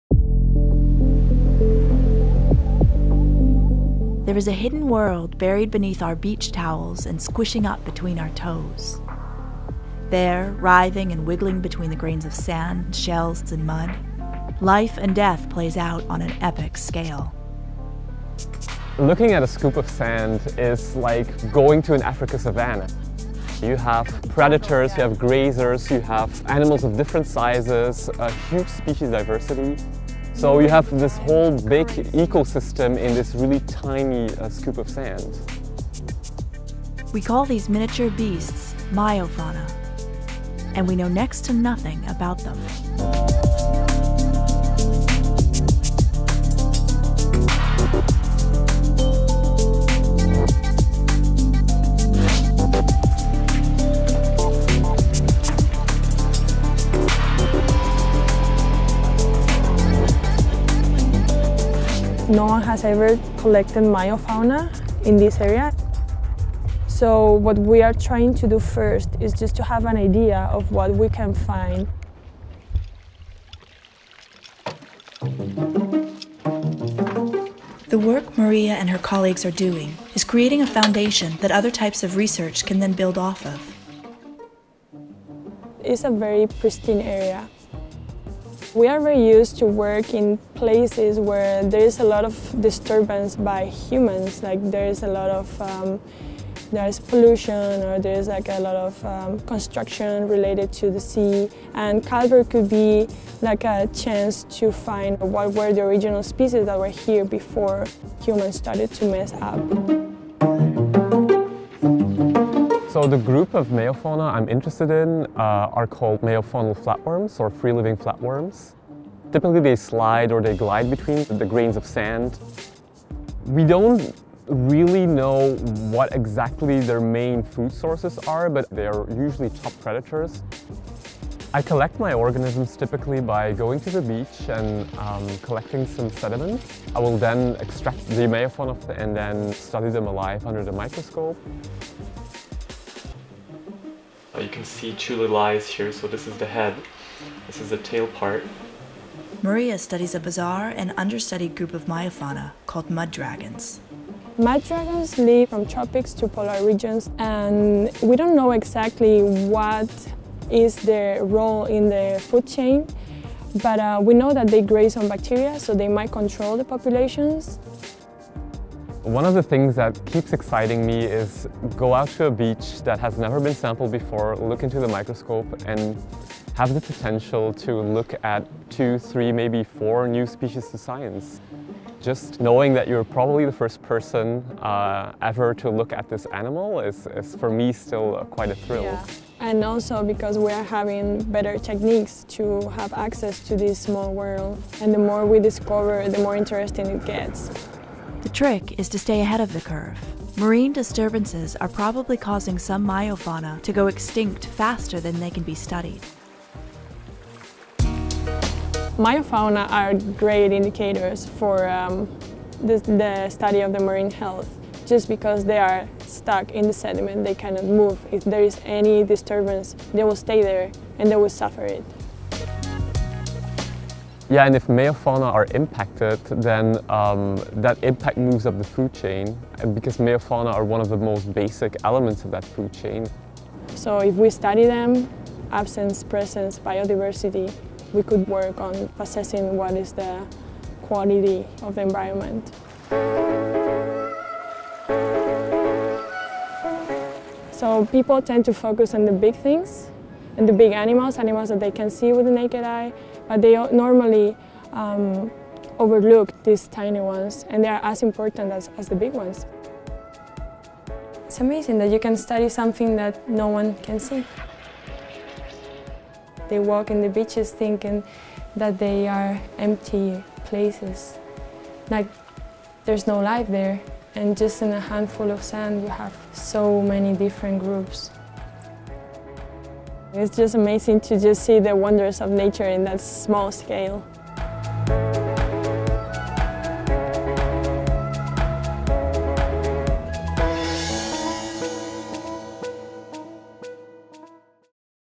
Here Be Tiny Dragons (and Other Micro Beasts)
Medals, titles, ribbons, a congratulatory hug from a parent—there are many ways to get accolades for our hard work. For biologists, one of the greatest honors is to discover a creature previously unknown to science. While it may seem that most living things have been discovered, it’s estimated that up to 91 percent of the world’s marine species still await description. Whales, birds, and fish are pretty well examined, but once you get past these charismatic megafauna and start looking at life in the oceans on a mini, even microscopic, scale, the possibility of seeing a creature that no other person ever has expands exponentially.

In this video, we meet two researchers who study meiofauna, creatures so small they live in between the grains of sand and mud. Like the creatures they study, the meiofauna research community is also small, which makes the potential for new discoveries almost endless. María Herranz and Neils Van Steenkiste, both postdocs at the University of British Columbia, take us to the domain of mud dragons and flatworms, nematodes and water bears. A walk on the beach will never be the same.

If you enjoyed this video, you’ll find lots of related stories on our You can sign up for our free weekly newsletter